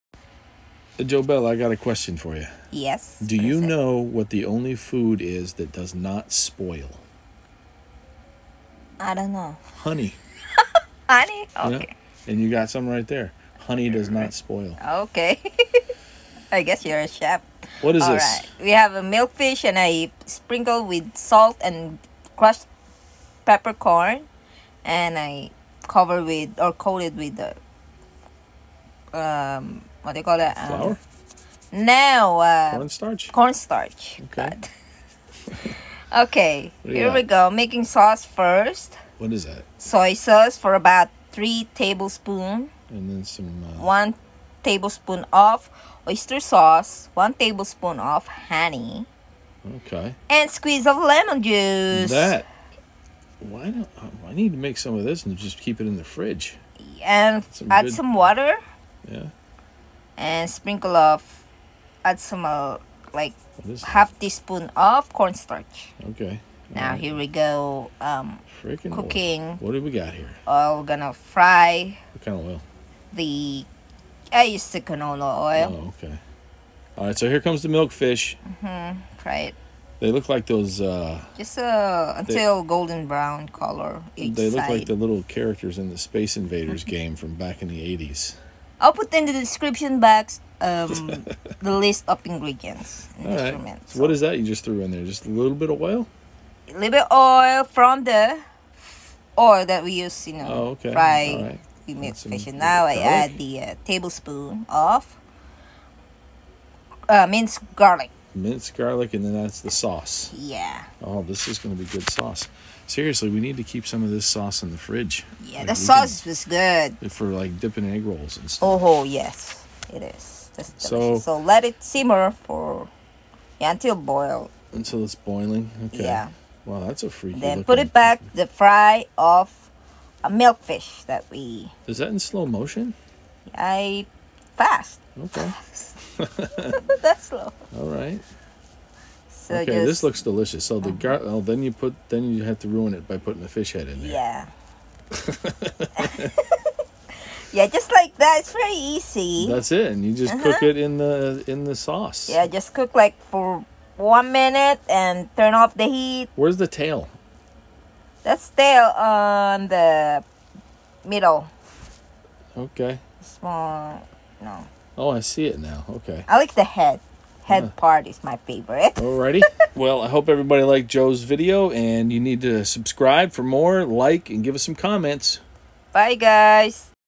Fried Milkfish in Oyster Sauce - Easy Recipe
Ingredients:
 1.5 Lbs Milkfish
3 Tbsp Soy Sauce
1 Tbsp Honey
1 Tbsp Oyster Sauce
1 Tbsp Minced Garlic
Salt and Crushed Peppercorn to taste.
Canola oil for cooking.